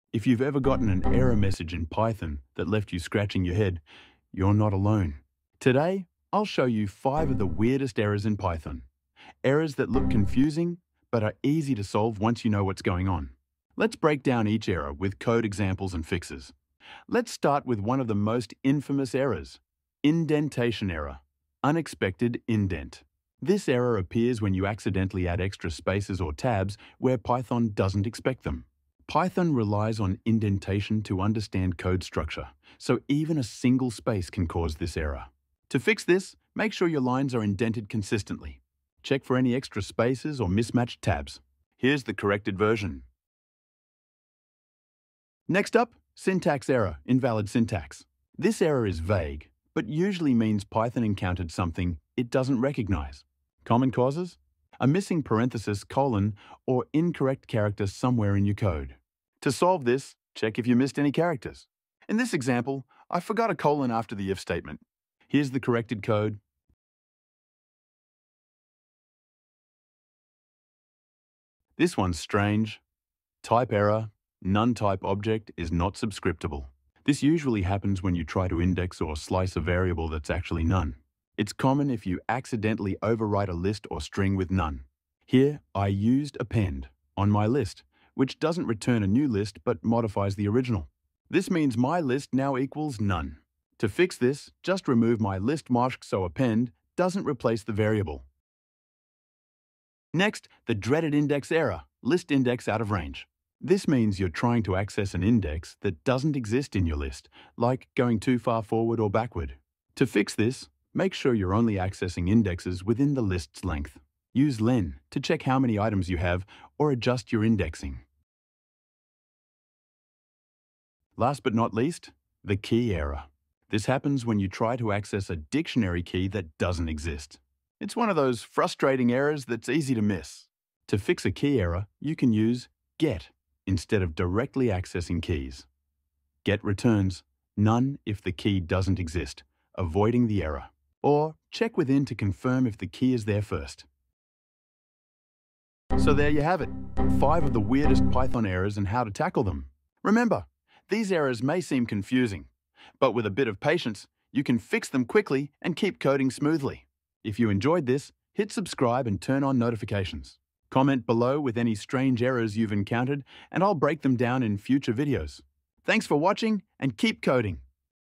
STOP Wasting Time on Python Errors You Can Fix in Seconds
Struggling with Python errors? In this video, we dive into the weirdest and most common Python errors, from IndentationError to KeyError, and guide you through fixing them step-by-step. Whether you're a beginner or an experienced coder, mastering these error-handling techniques will boost your Python skills and help you code with confidence. Learn how to troubleshoot and resolve Python exceptions quickly, with clear explanations and practical tips to keep your code running smoothly!